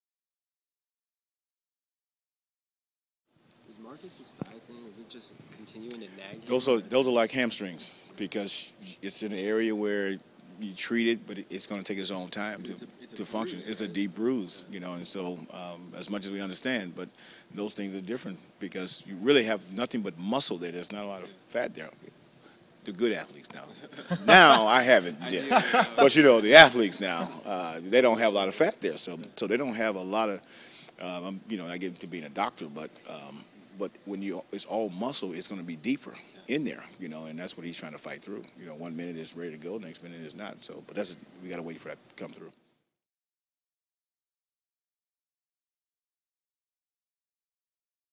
Keith Smart on Marcus Thornton's thigh injury
Following shootaround at the Kings practice facility today, Sacramento Kings head coach Keith Smart addressed guard Marcus Thornton's thigh injury with media.